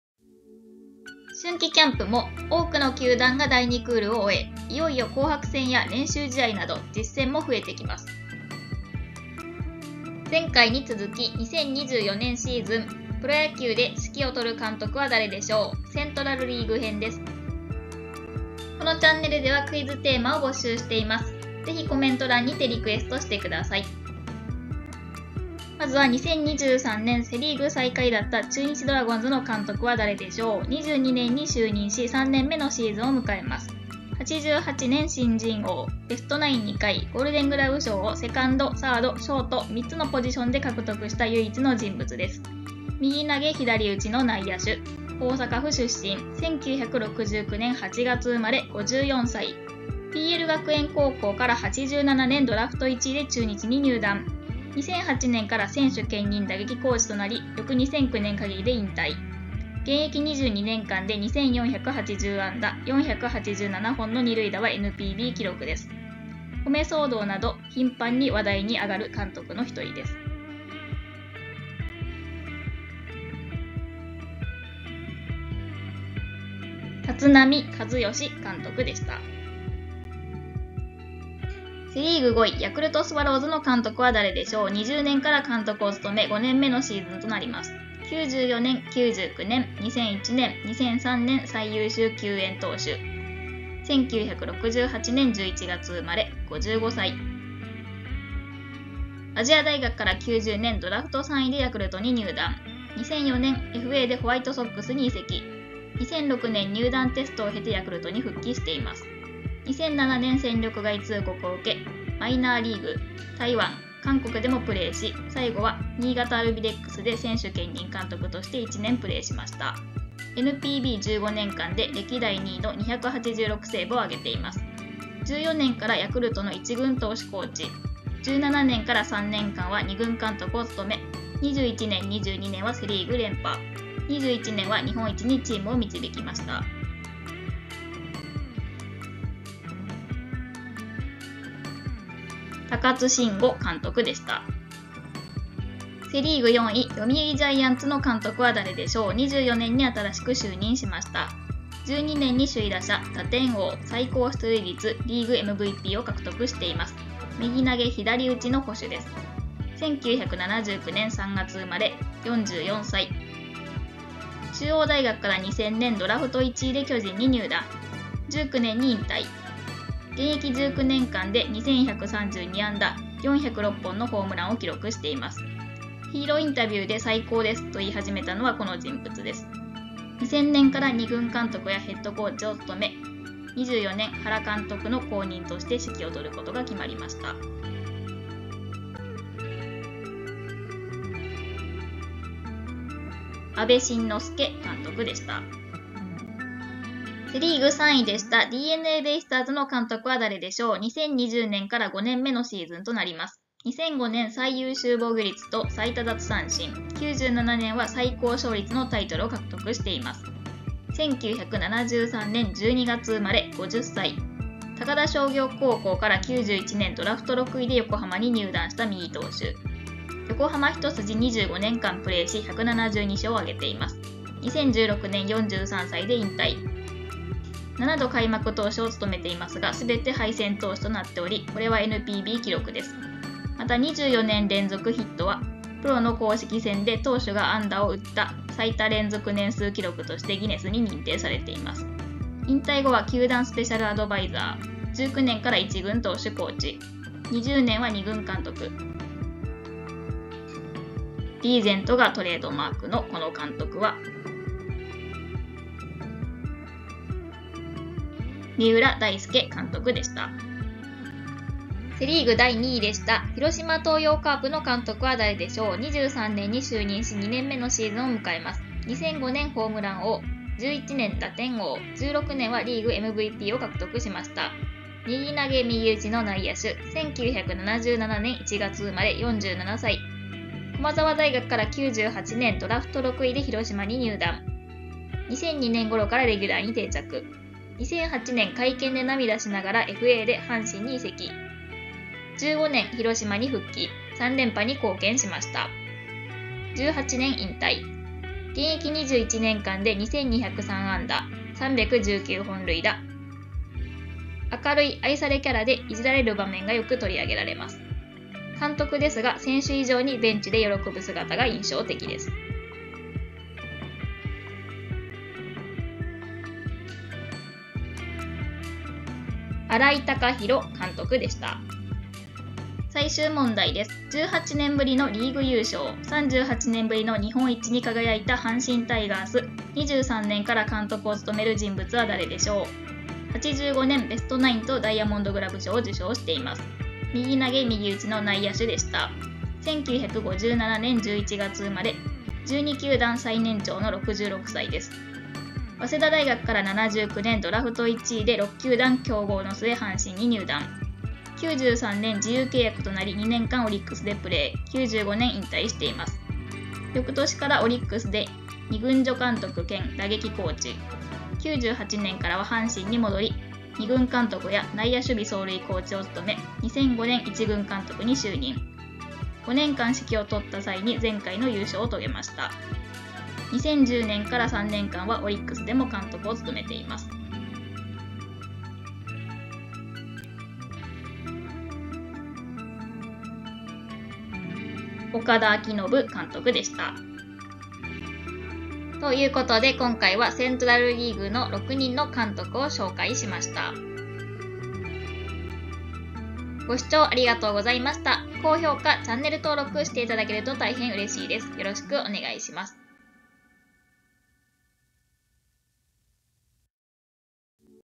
現役時代はこんな選手でした！セリーグ監督DEクイズ
野球に関するクイズを出題するチャンネルです⚾
動画の先行公開、ランキングの続きや限定動画もあります✨

今回のテーマは、「監督」
データは2024年2月6日現在のものです。

セントラルリーグ監督6人をクイズにしました。
あなたは何人わかりますか(*^O^*）

☆共通点多数あり！
☆選手時代のタイトル、ポジション、経歴から当ててください。

特集してほしいテーマ募集中！